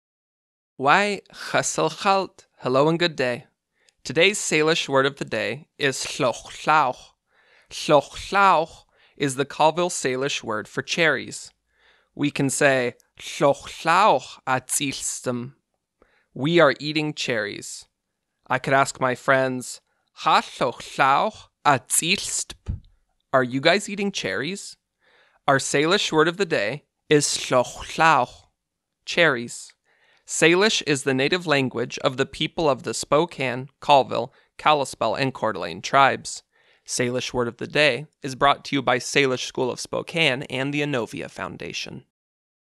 Salish Word of the Day No. 145 łəx̌ʷłax̌ʷ / cherries
iʔ n̓səl̓xčin̓ sʔum̓s iʔ k̓l̓ ʕapnáʔ t sx̌l̓x̌ʕal̓t, sʔaslásq̓t, 29 sq̓ʷsus, 2019: łəx̌ʷłax̌ʷ.

Colville Salish word of the day for Tuesday, January 29, 2019: cherries